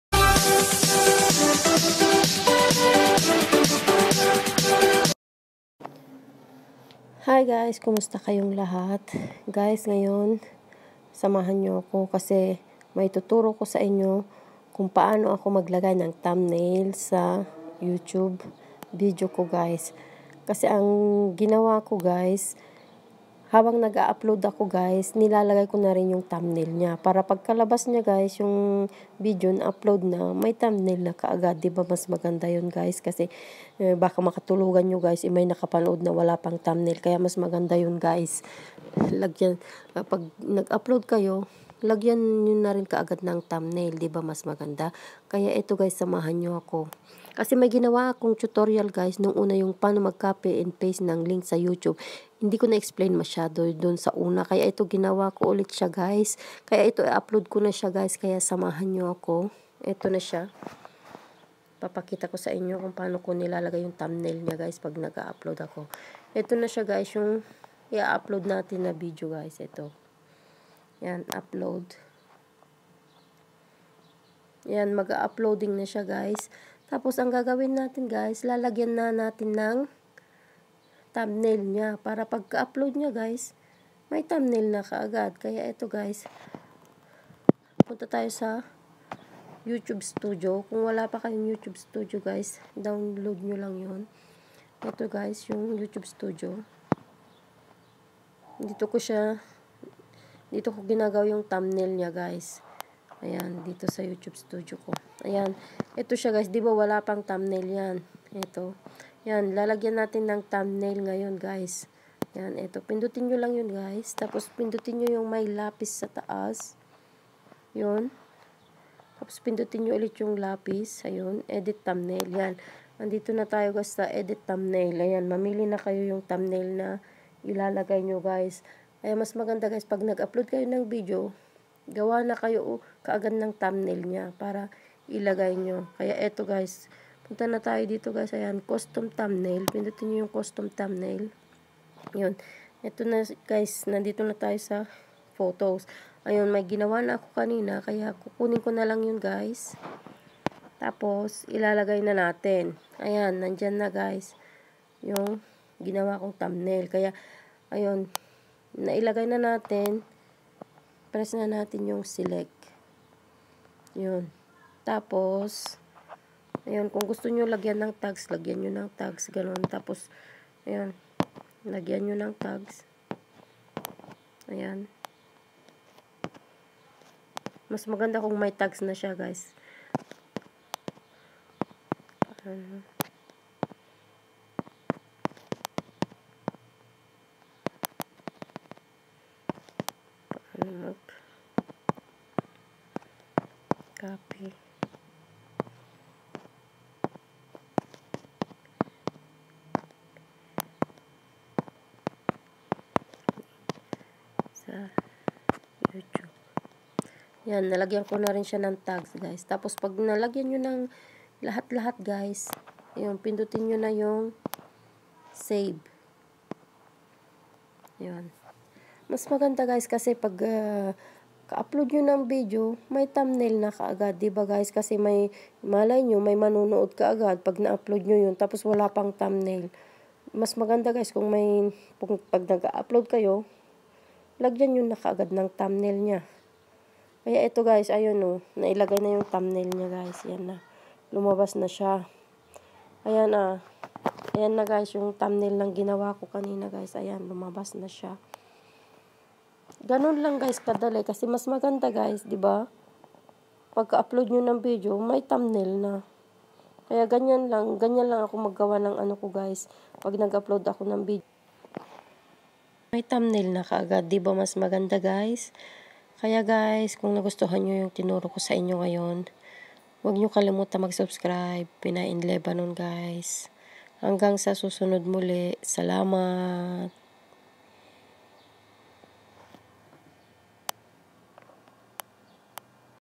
PAANO MAGLAGAY NG THUMBNAIL GAMIT ANG YOUTUBE STUDIO
Hi guys kung gusto ni nyo malaman  kung paano maglagay ng thumbnail gamit ang youtube studio panoorin nyo ang video ko guys.mas maganda kc habang nag aapplod yong video mo lag yan muna rin ng thumbnail para sabay sila guys.